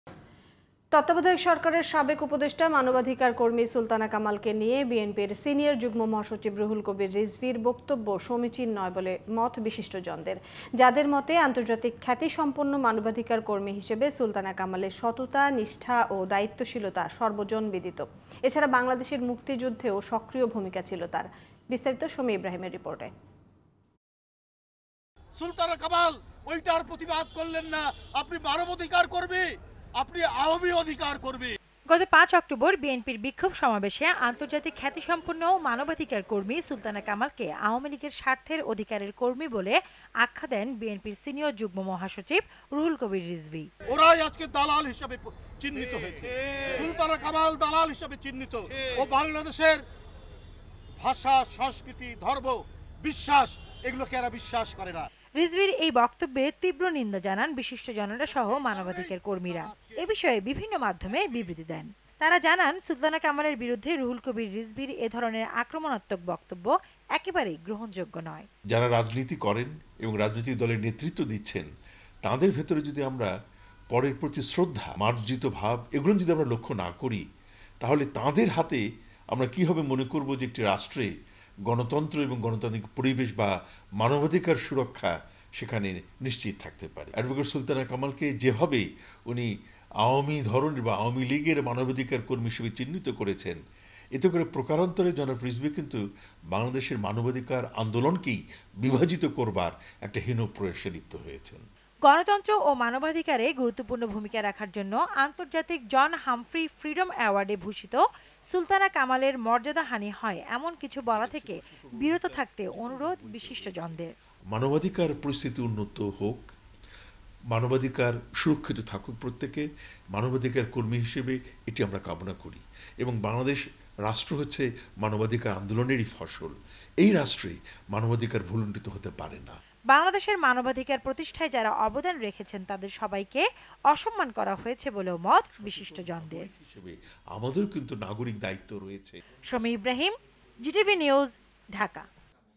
সুলতানা কামালকে নিয়ে রুহুল কবির রিজভীর বক্তব্যের সমালোচনা করেছেন বিশিষ্টজনরা | Sultana Kamal
সুলতানা কামালকে নিয়ে রুহুল কবির রিজভীর বক্তব্যের সমালোচনা করেছেন বিশিষ্টজনরা | Sultana Kamal | 08Oct2022